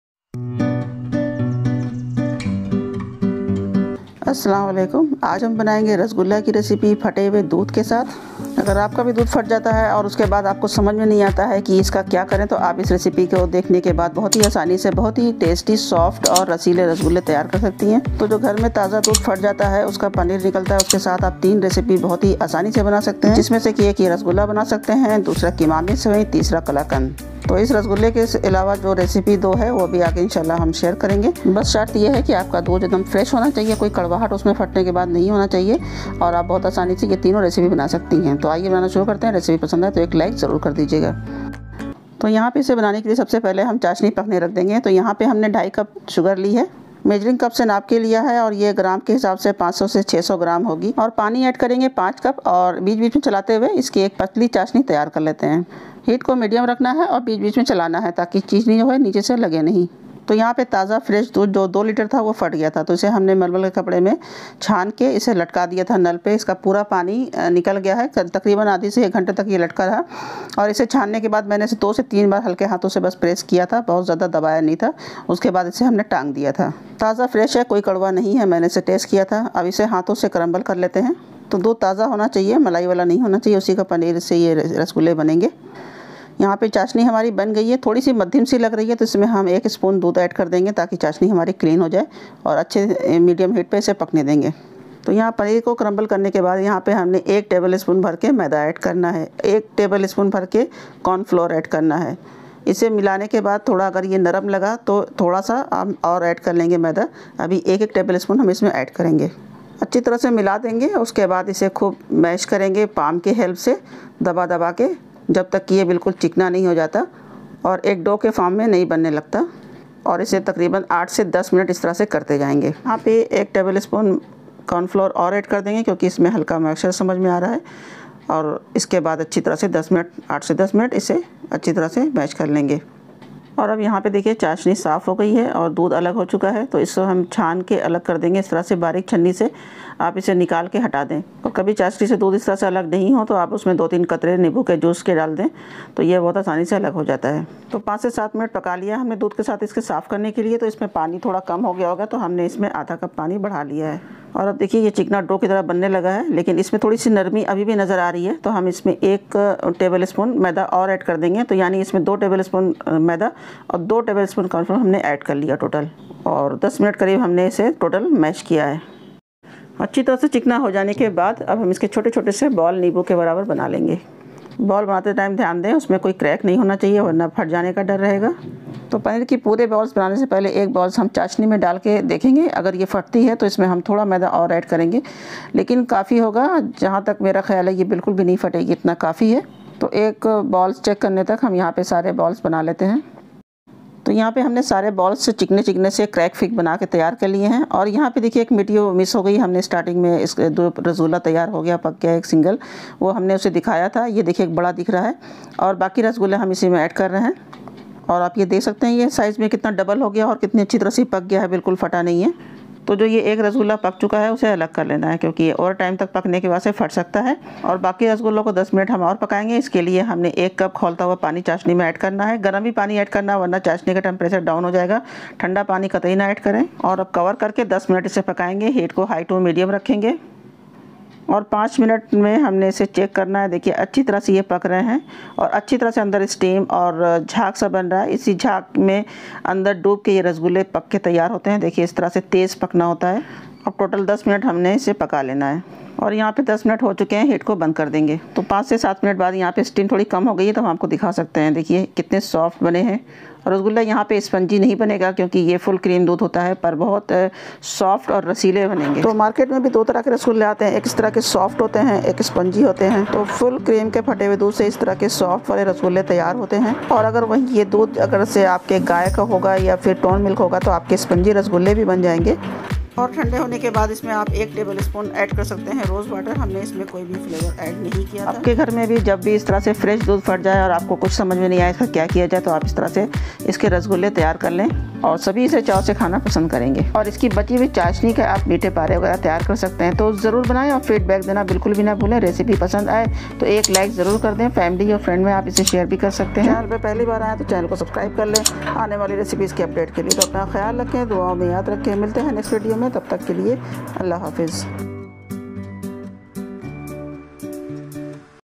Curdled milk (Fate Doodh)se banaye perfect soft rasgulle❤️ By Zaika-e-Lucknow
📍 Ingredients:-

• Milk
• Sugar 2.5 cups / 500-600 gm
• Refined flour 2 tbsp